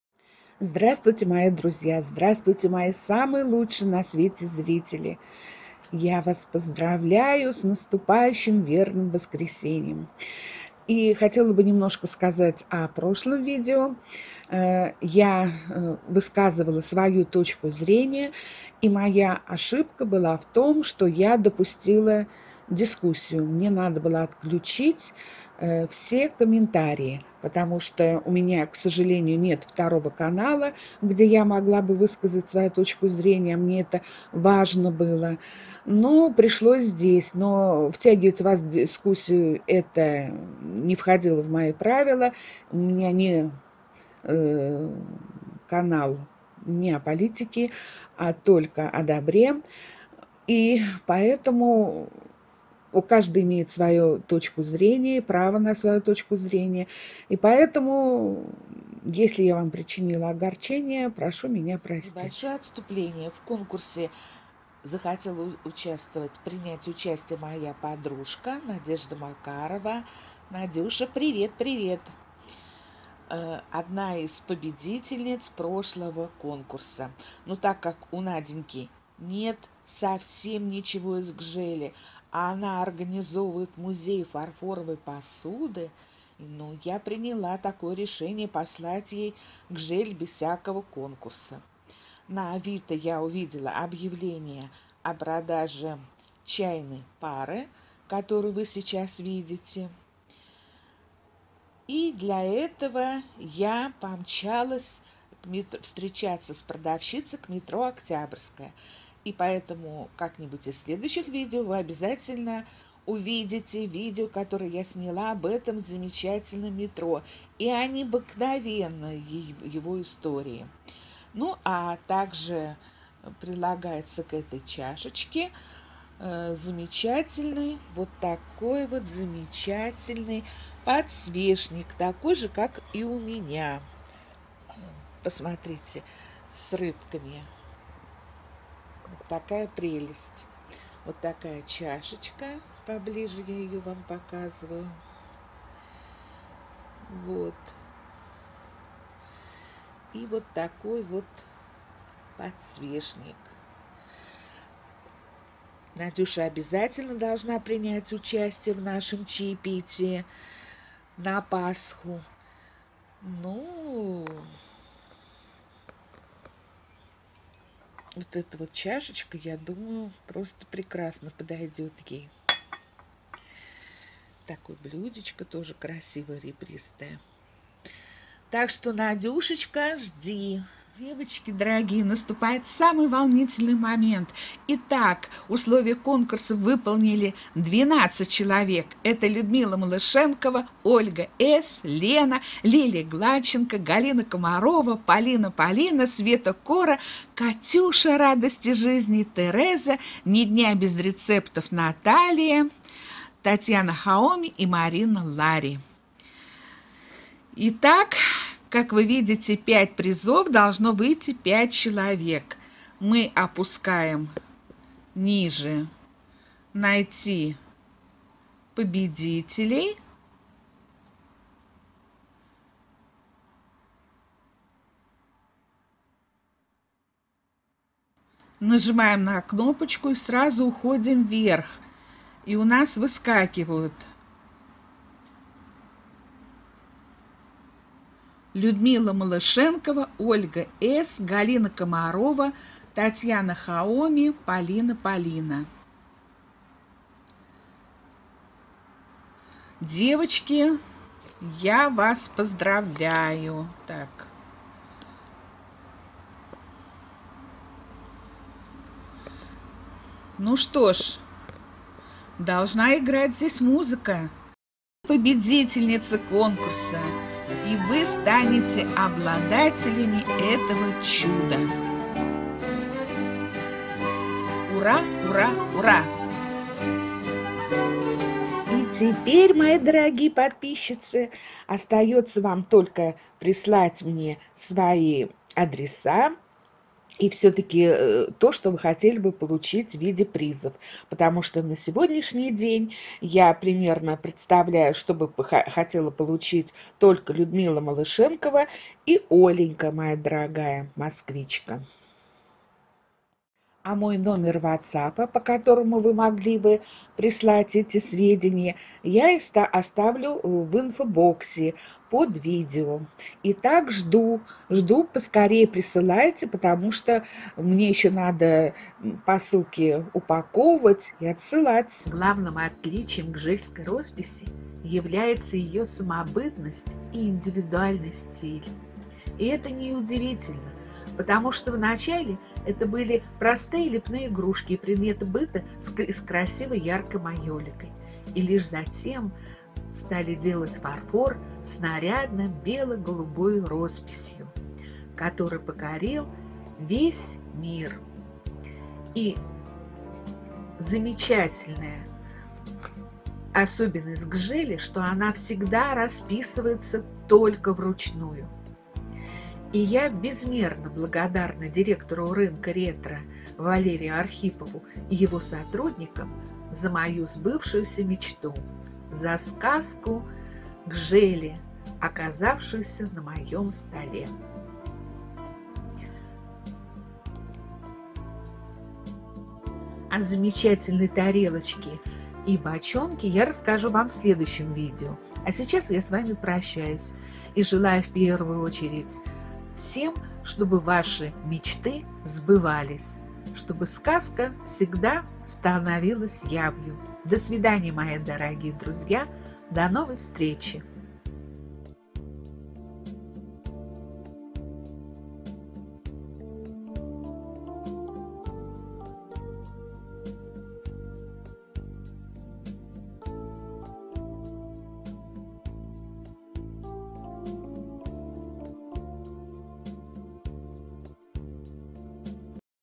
Победители конкурса.!💖🌹🌷!💖🌹🌷!💖🌹🌷Антиквариат СССР. Винтаж. Гжель.
Поздравляю победителей конкурса:Людмила Малашенкова,Оленька С.,Галина Камарова, Татьяна Х Киев, Полина. Полина.
Мой телефон ВАЦАПА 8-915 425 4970